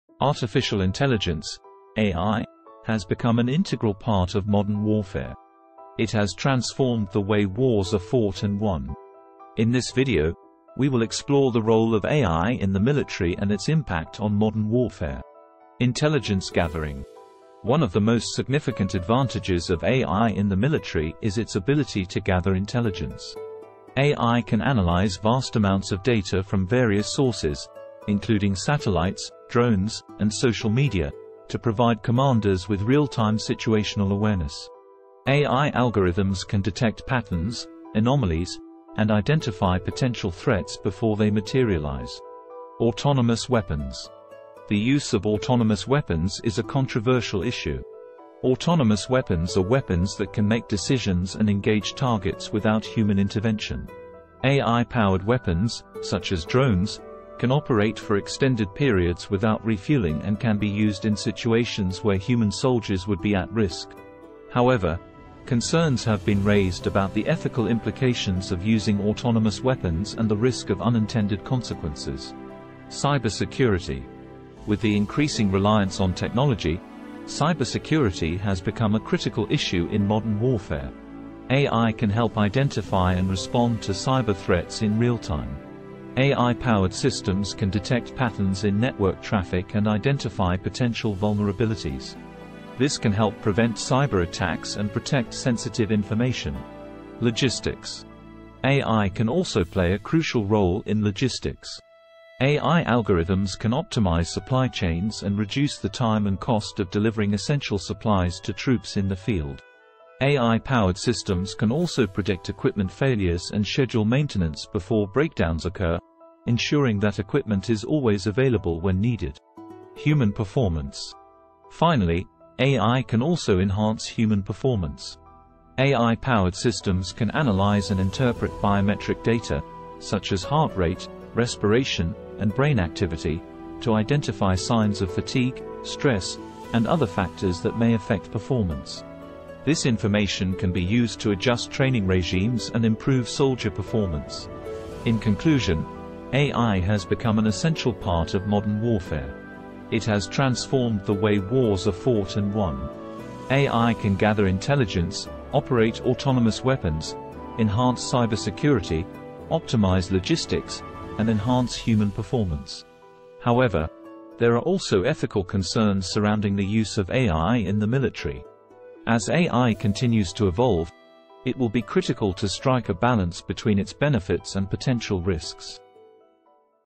The role of AI in the military [1.20.]
Artificial intelligence (AI) has become an integral part of modern warfare.

It has transformed the way wars are fought and won.

In this video, we will explore the role of AI in the military and its impact on modern warfare.

Artificial intelligence
AI
Survive AI

The role of AI in the military

Intelligence Gathering
Autonomous Weapons
Cybersecurity
Logistics
Human Performance